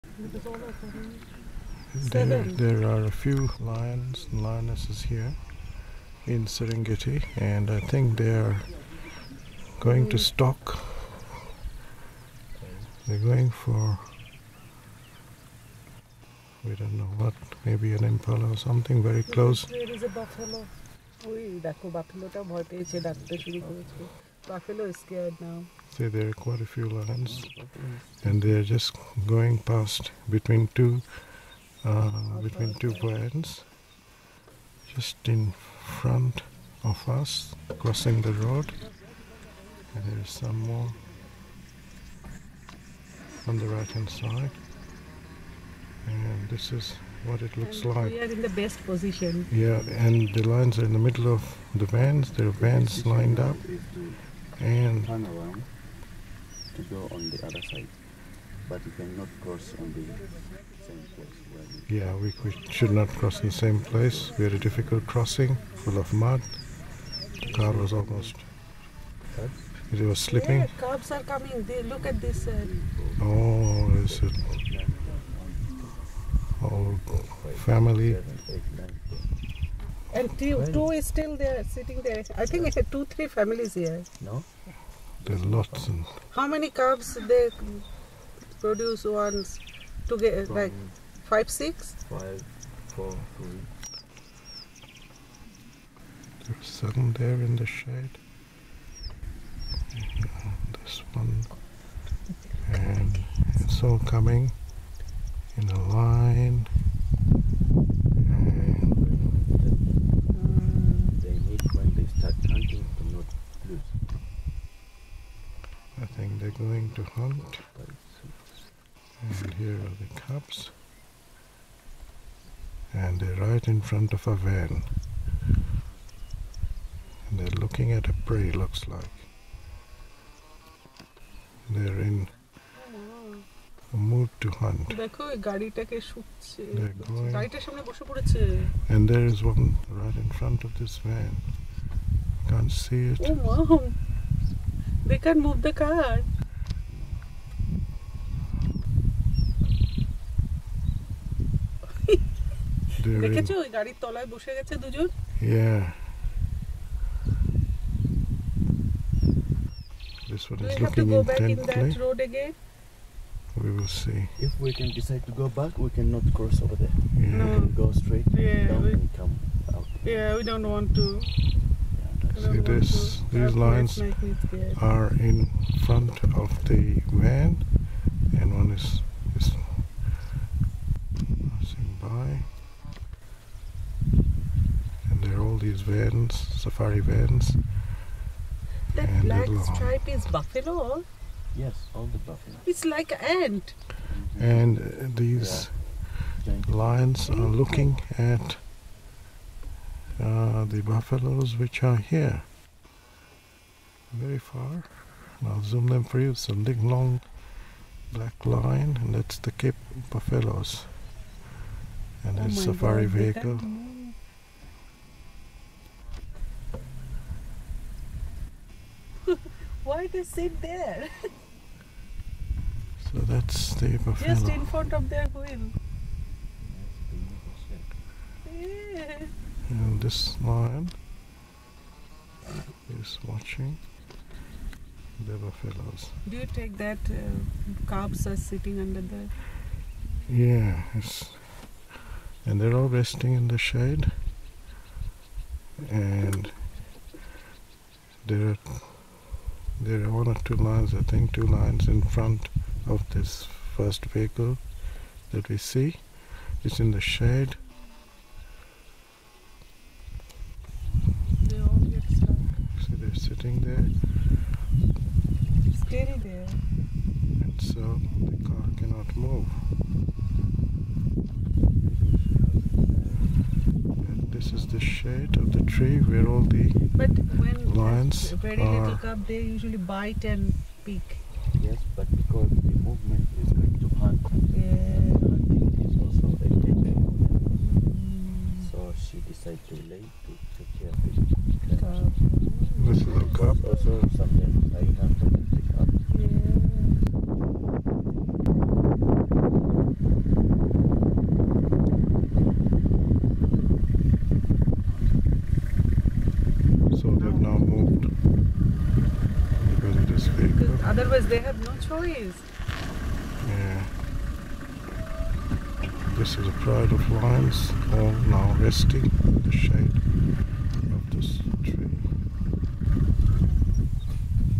Serengeti Lions
Lions in Serengeti NP near our safari jeep during our trip in Jan/Feb 2015